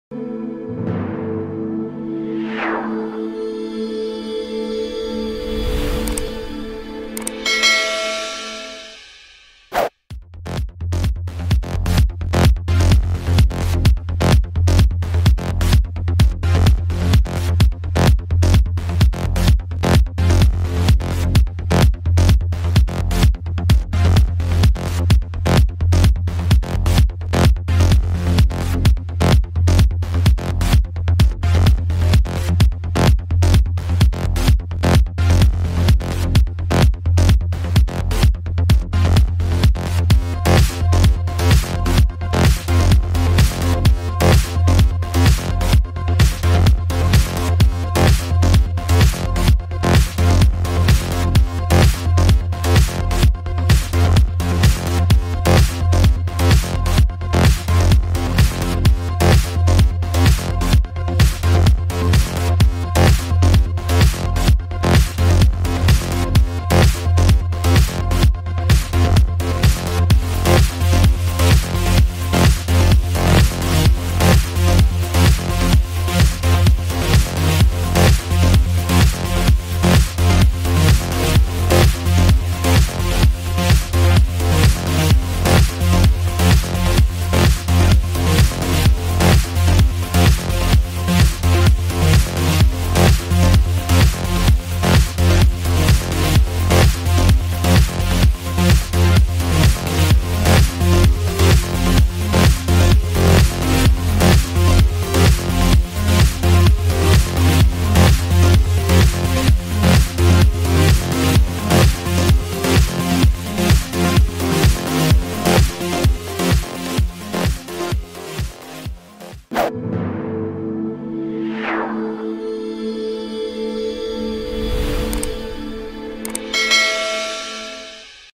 අඩි 12000ක සිට පහලට ... Skydiving from 12000 feet
අඩි 12000ක  සිට පහලට ...
Skydive Wanaka in the South Island of New Zealand